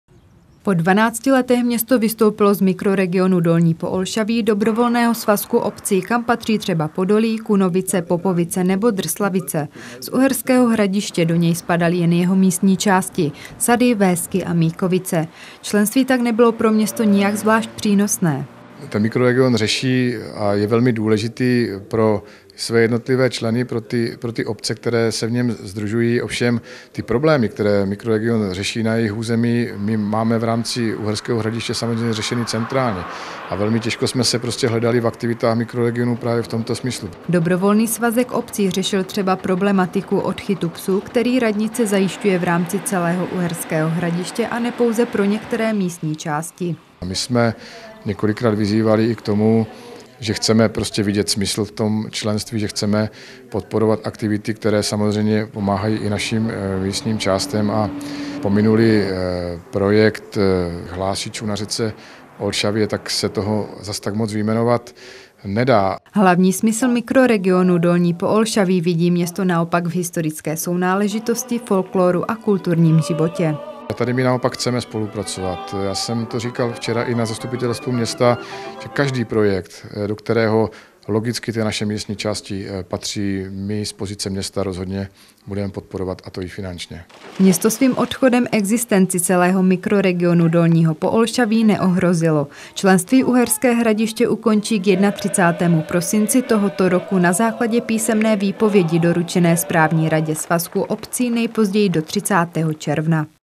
TVS: Uherské Hradiště - Mikroregion Dolní Poolšaví
Uherské Hradiště ukončilo členství v Mikroregionu Dolní Poolšaví, které se městu nejevilo jako přínosné. Součástí dobrovolného svazku obcí bylo od roku 2005.